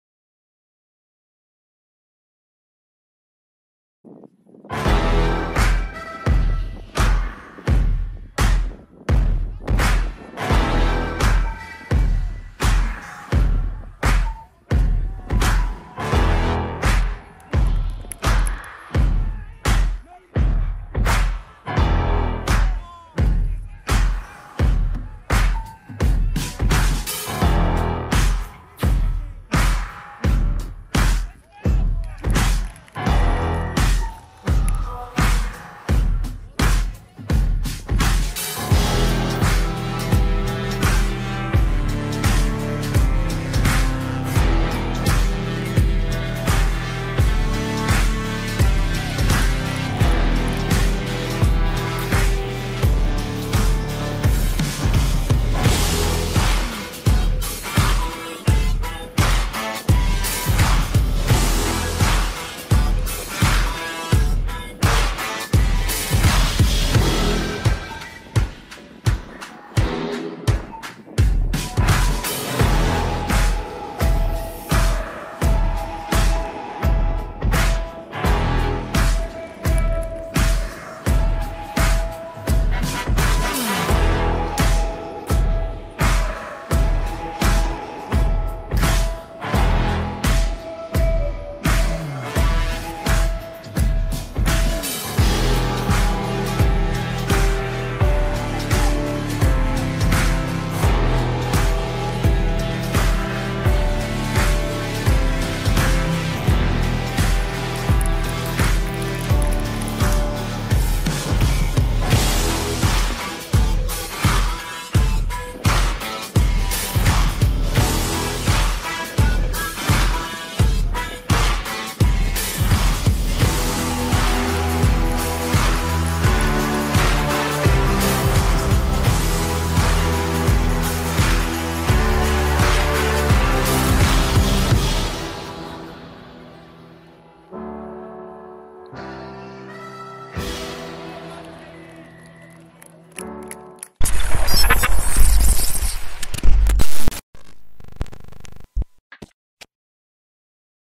TryLights | Colts XV 50-5 Chess Valley | 2023:24
TryLights from our Colts XV clash with Chess Valley Colts | 15.10.23